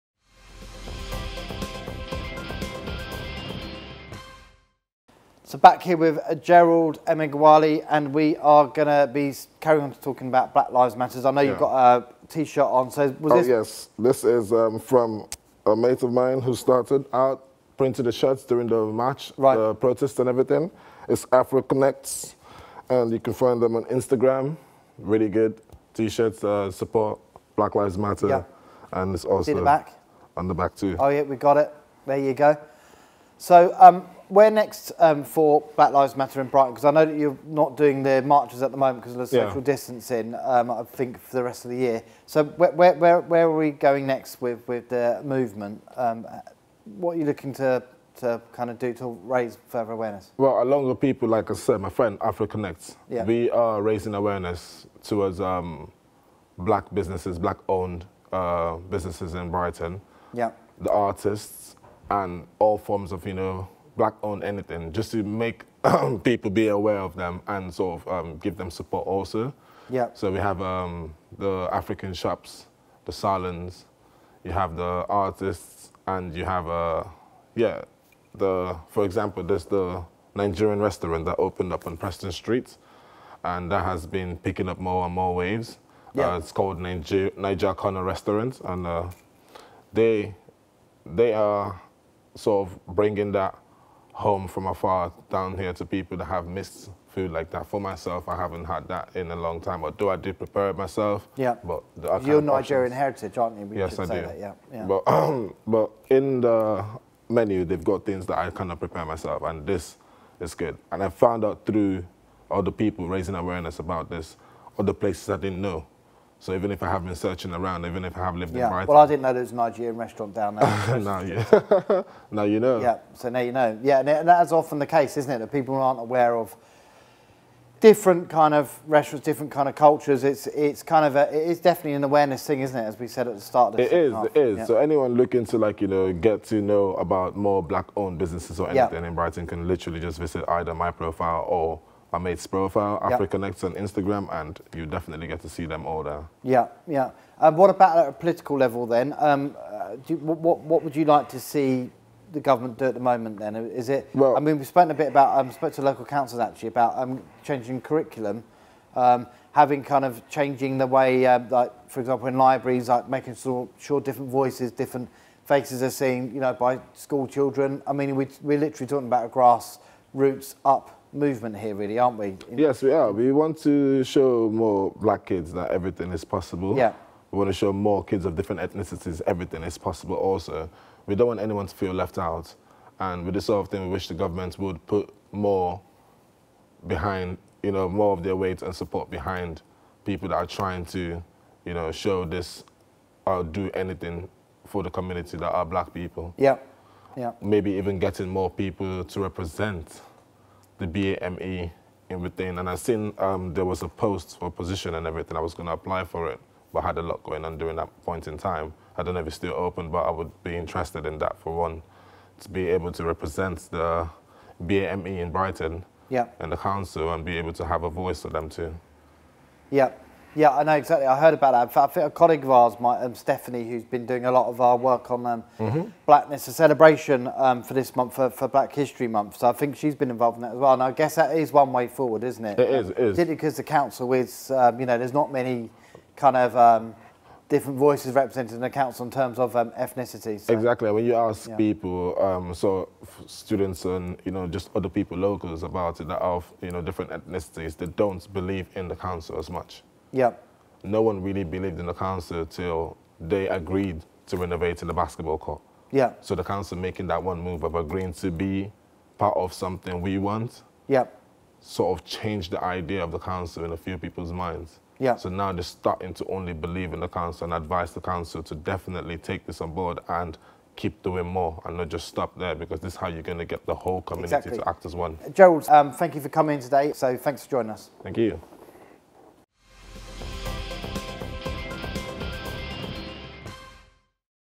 Gerrald Emeagwali encourages the community to act as one for the BLM movement!
Latest TV News Editor, Mark Walker, speaks to Gerrald Emeagwali about what's next for the Black Lives Matter movement. Gerrald discuss raising awareness for local black owned businesses and representing BME groups within Brighton.